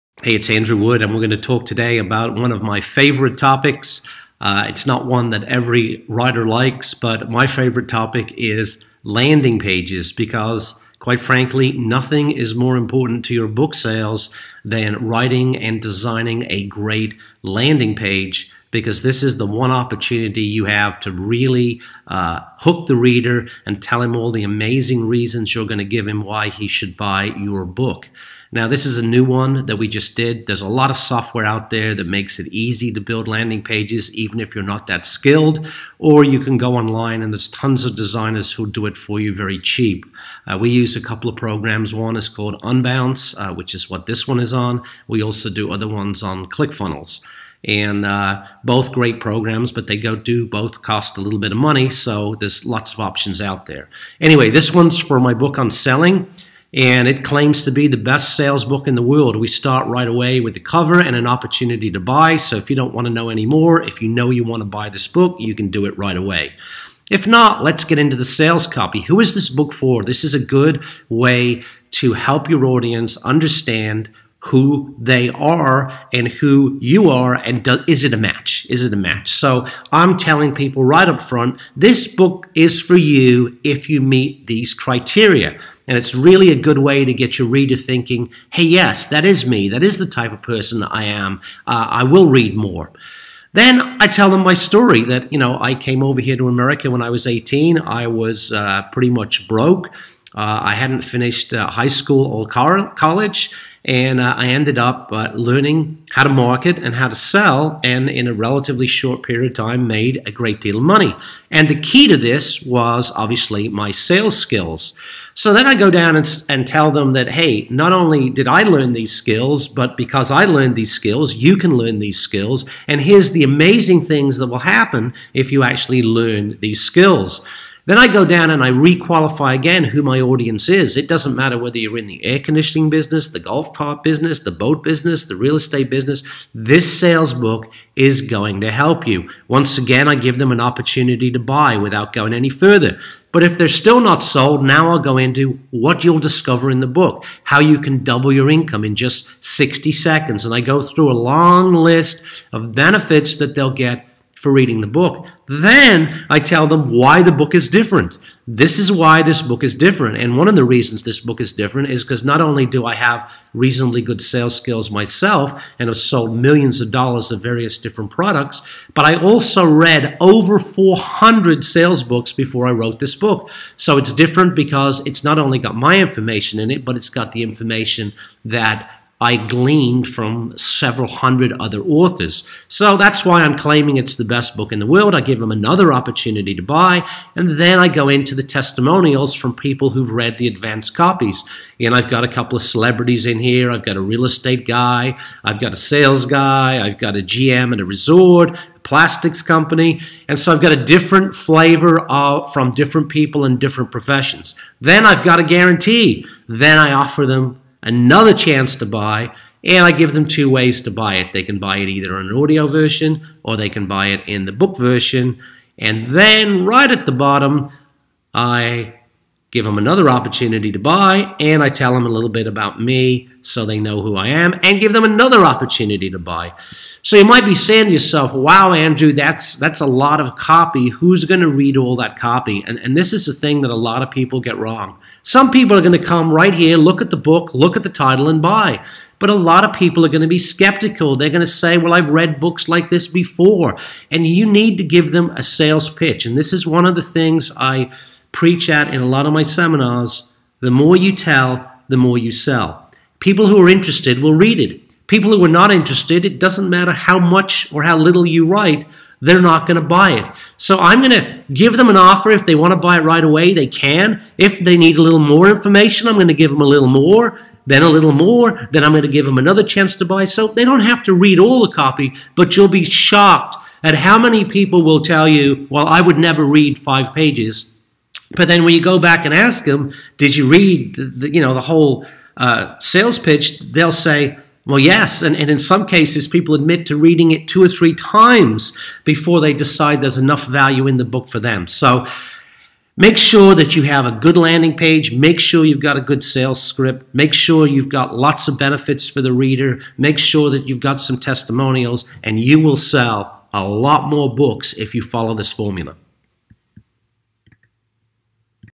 Increasing Book Sales Through Landing Pages!
Hi, this video about increasing book sales through landing pages.
Here is everything you need to know about designing your landing pages to drive your book sales through the roof!

Do you have any question about this video? If you have any tips about book marketing, Please share with us.